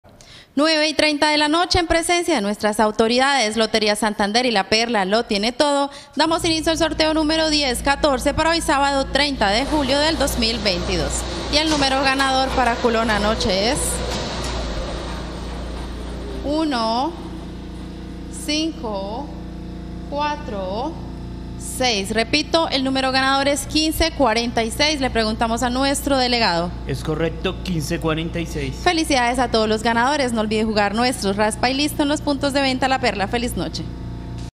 CULONA NOCHE Resultado del SÁBADO 30 DE JULIO de 2022 ✅🥇🔥💰
Hola buenas noches, te presentamos los resultados de las loterías y chances en Colombia, vamos con el Resultados del Chance Culona noche del SÁBADO 30 DE JULIO de 2022.

Resultado Extra de Colombia SÁBADO 30 DE JULIO de 2022